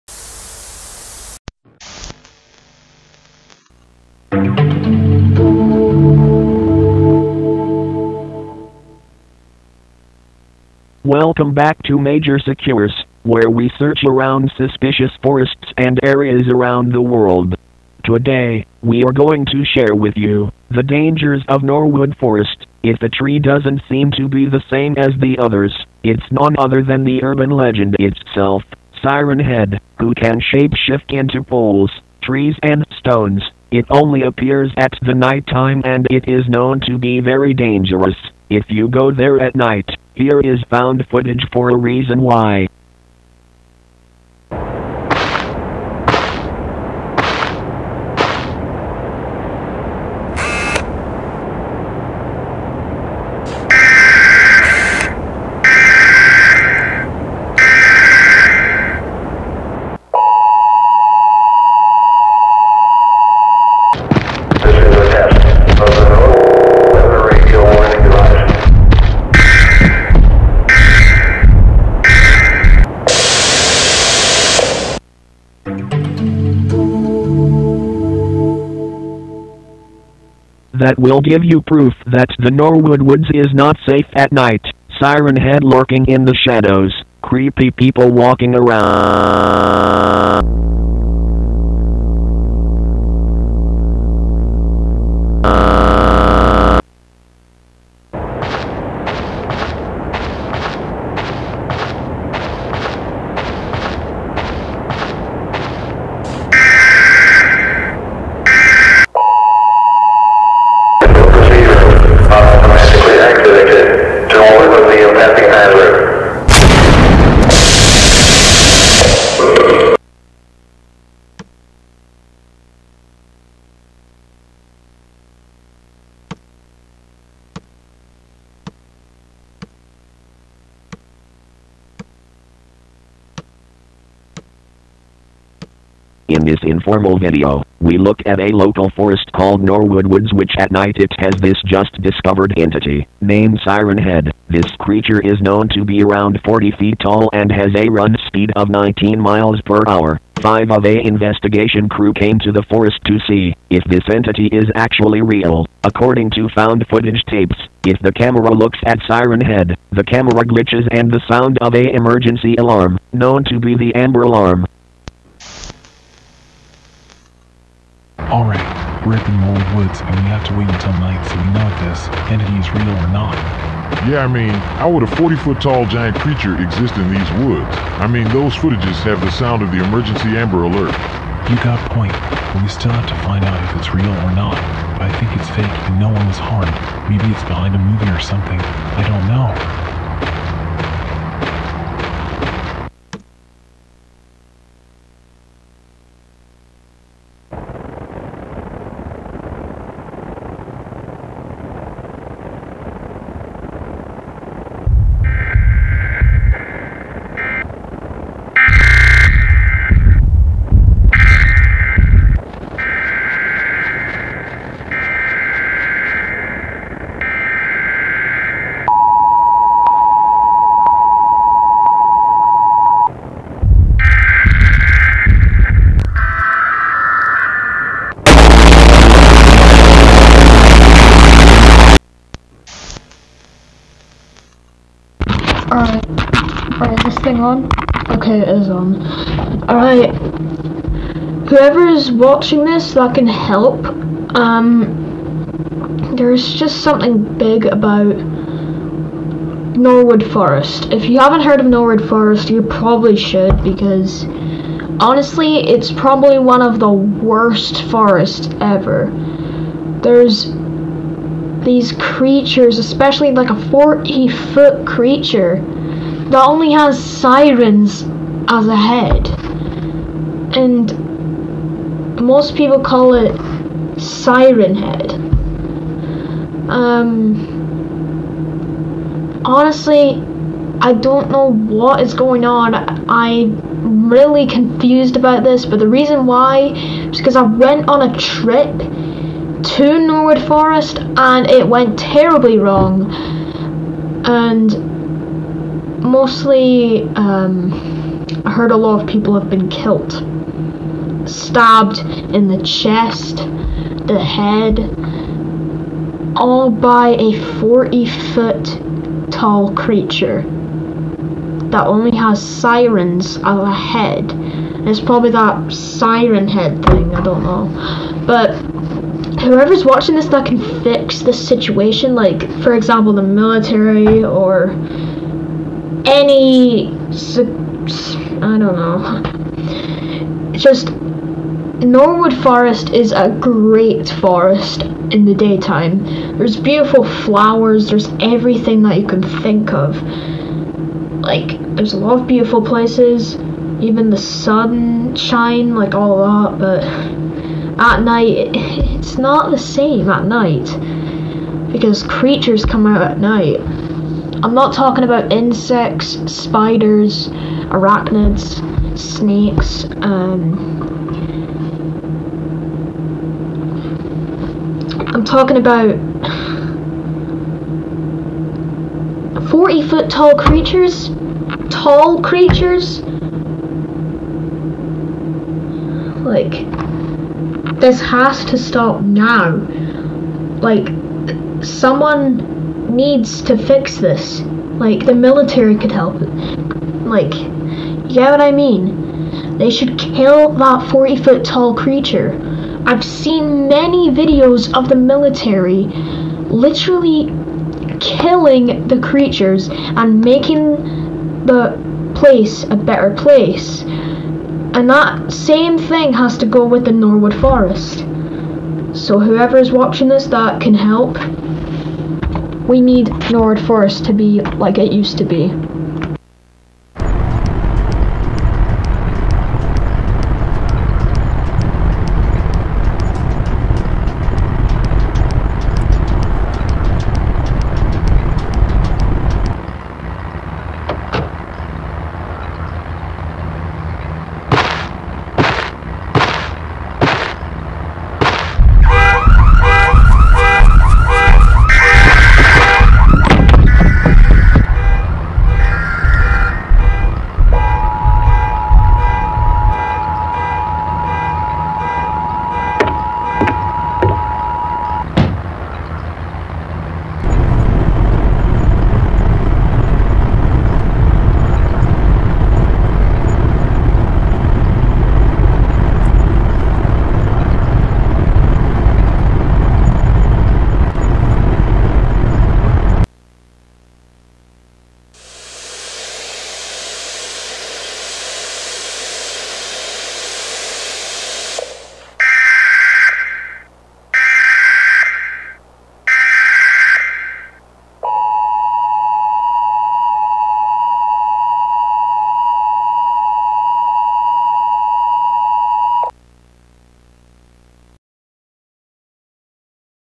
The Siren Head Tapes Vol. 1
Siren Head Is Going Insane, The Military Needs To Stop Him!

WARNING: This Video Contains Flashing Lights, Loud Noise And Disturbing Scenes

This Video Is Allowed In Reaction Videos :)

Softwares Used:
- Source Filmmaker
- Adobe Premiere Pro
- NTSCQT (VHS EFFECT)

All Inspired By:
- Modus Interactive